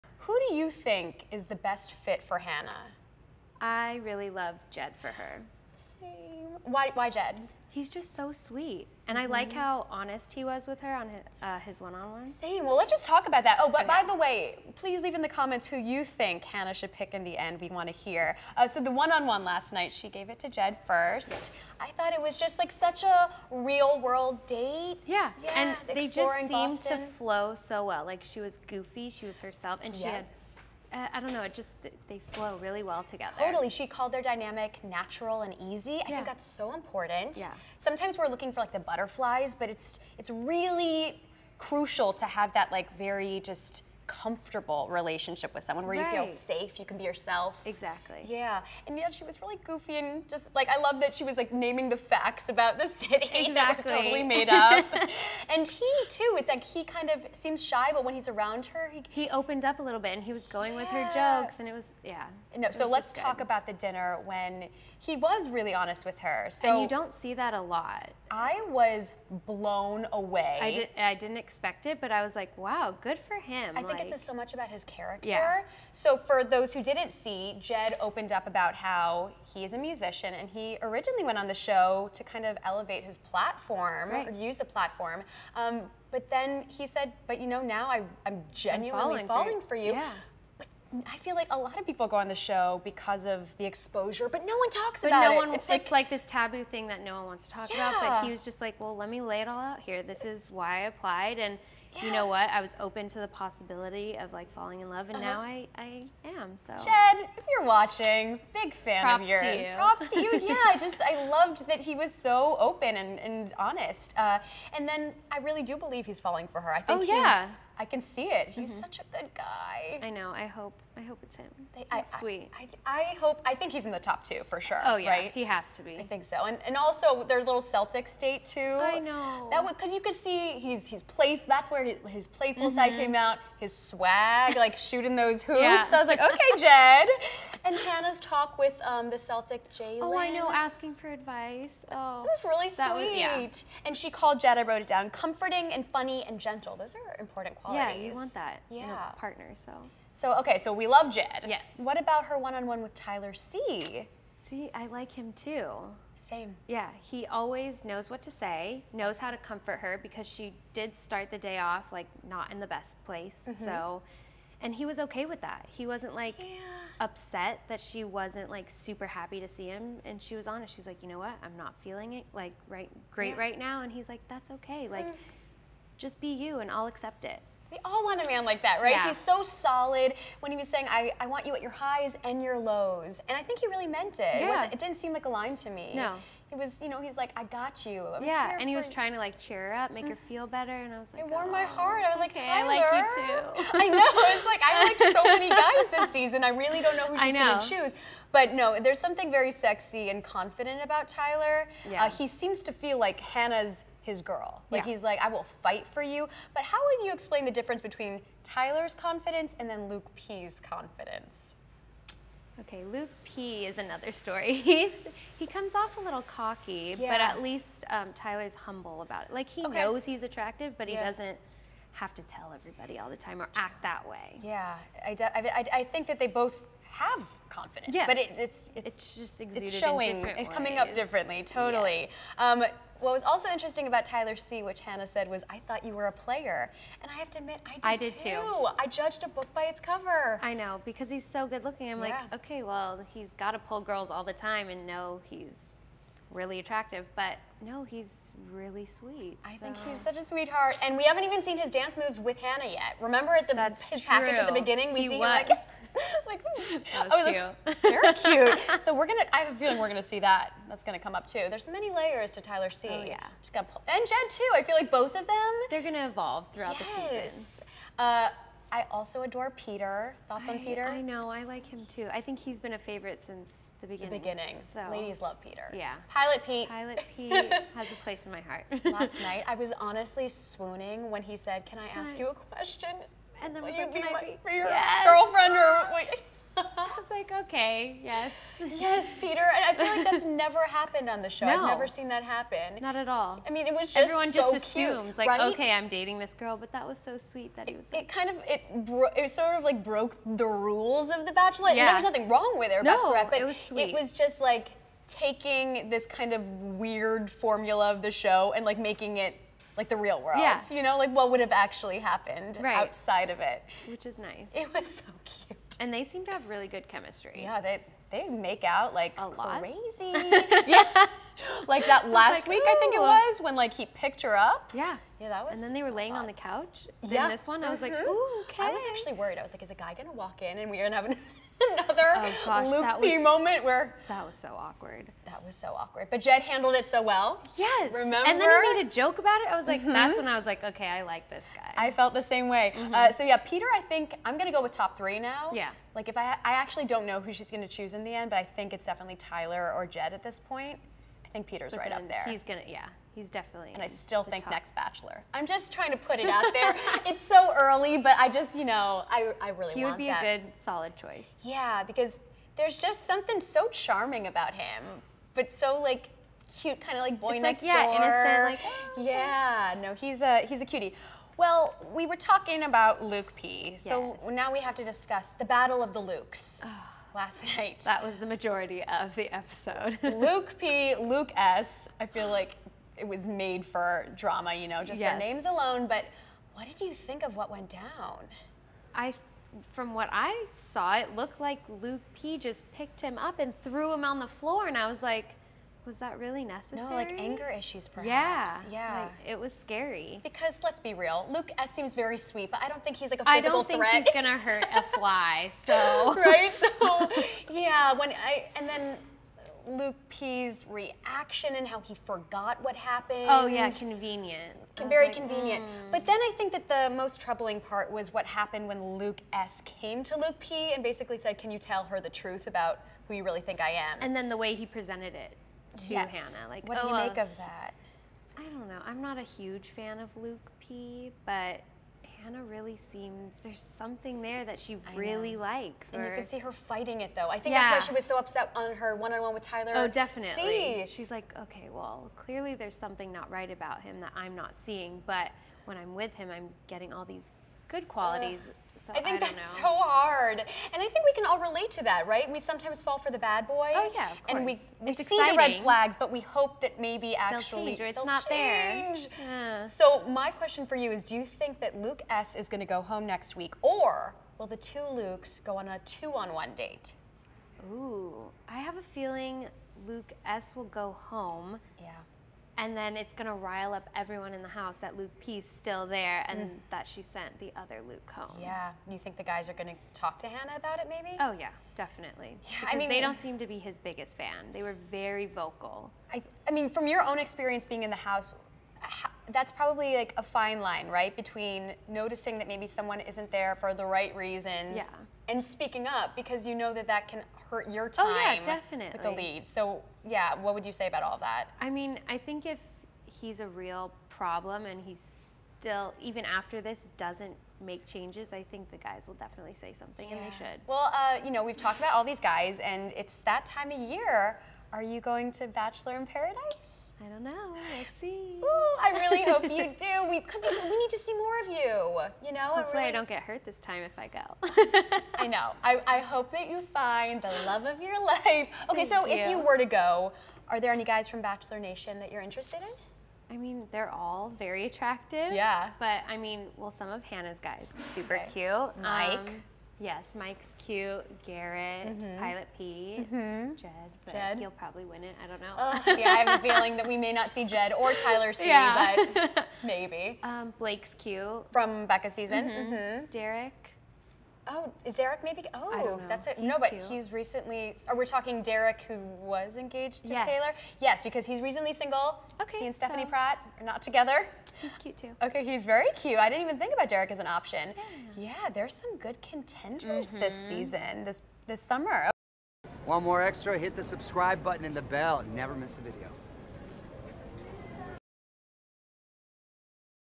‘The Bachelorette’: The Battle of the Lukes! Plus: Jed's Big Confession
“The Bachelor” alum Kirpa Sudick joined “Extra” for the latest edition of our “Bachelorette” Breakdown! We dished on Hannah B.’s one-on-one dates with Jed and Tyler C., along with the major drama between Luke P. and Luke S.! Plus, he had to find out — will Kirpa be going to “Bachelor in Paradise”?